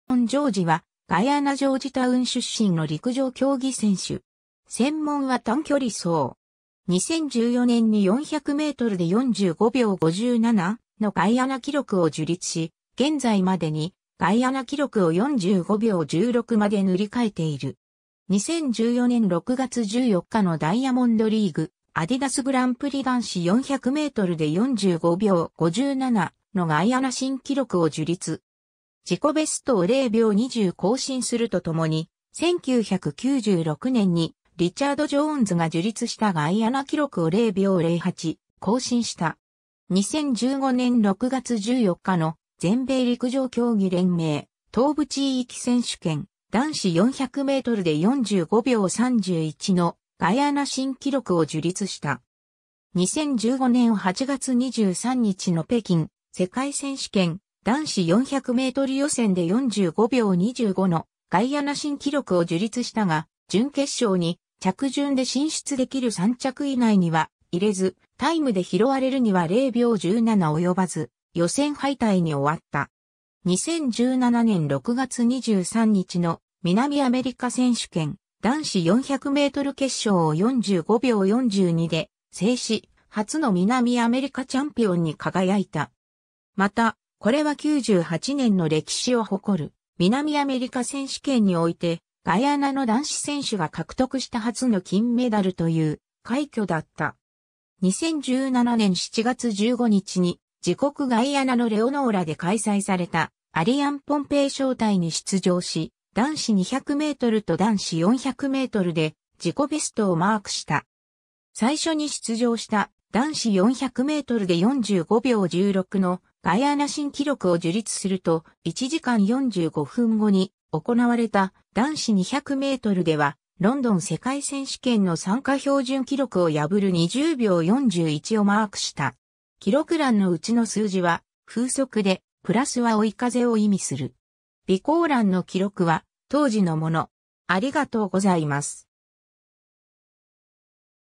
ウィンストン・ジョージ
ガイアナ・ジョージタウン出身の人物
ウィンストン・ジョージ（Winston Oudkerk George、1987年5月19日 ‐ ）は、ガイアナ・ジョージタウン出身の陸上競技選手。
2014年に400mで45秒57のガイアナ記録を樹立し、現在までにガイアナ記録を45秒16まで塗り替えている。
 2014年6月14日のダイヤモンドリーグ・アディダスグランプリ男子400mで45秒57のガイアナ新記録（当時）を樹立。
自己ベストを0秒20更新するとともに、1996年にリチャード・ジョーンズ（Richard Jones）が樹立したガイアナ記録を0秒08更新した。
 2015年6月14日の全米陸上競技連盟・東部地域選手権（United States of America Track and Field federation's Eastern Regional Championships）男子400mで45秒31のガイアナ新記録（当時）を樹立した。
 2015年8月23日の北京世界選手権男子400m予選で45秒25のガイアナ新記録（当時）を樹立したが、準決勝に着順で進出できる3着以内には入れず、タイムで拾われるには0秒17及ばず予選敗退に終わった。
 2017年6月23日の南アメリカ選手権 (en) 男子400m決勝を45秒42で制し、初の南アメリカチャンピオンに輝いた。
また、これは98年の歴史を誇る南アメリカ選手権において、ガイアナの男子選手が獲得した初の金メダルという快挙だった（全ての種目を通じて）。
 2017年7月15日に自国ガイアナのレオノーラで開催されたアリアン・ポンペイ招待（Aliann Pompey Invitational）に出場し、男子200mと男子400mで自己ベストをマークした。
最初に出場した男子400mで45秒16のガイアナ新記録を樹立すると、1時間45分後に行われた男子200mではロンドン世界選手権の参加標準記録（20秒44）を破る20秒41（+0.1）をマークした。
 記録欄の（　）内の数字は風速（m/s）で、+は追い風を意味する。
 備考欄の記録は当時のもの